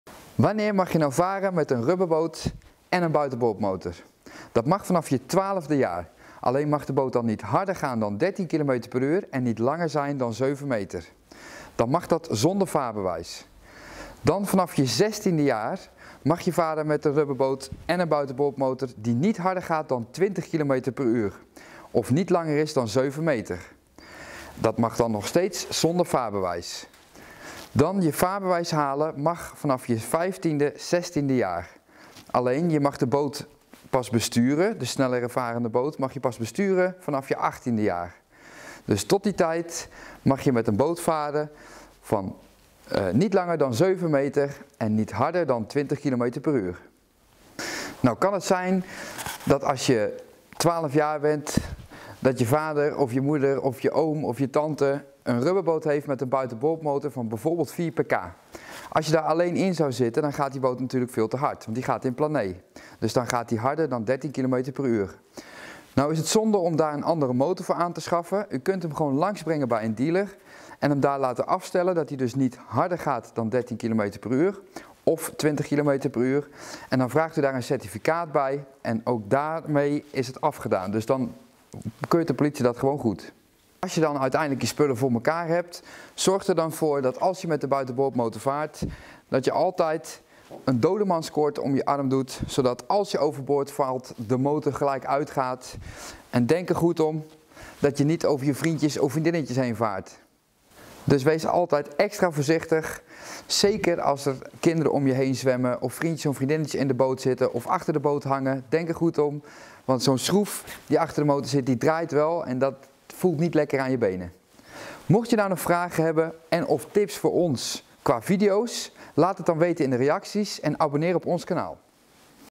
HOE OUD MOET JE ZIJN VOOR EEN RUBBERBOOT - TIPS MET DE BOTEN DOKTER
Hoe oud moet je zijn om te varen met een rubberboot
Vanaf welke leeftijd mag je varen met een rubberboot met motor?
Wanneer heb je een vaarbewijs nodig,
Die vraag krijgen wij vaak. Vandaar dat wij hier een video over hebben gemaakt.
Vanaf je 12de jaar mag je een boot met motor besturen. Maar dan mag hij niet langer dan 7 meter zijn en niet harder kunnen dan 13km per uur.

Mochten Jullie filmpjes willen zien, die hier nog niet staan? Laat het ons dan weten.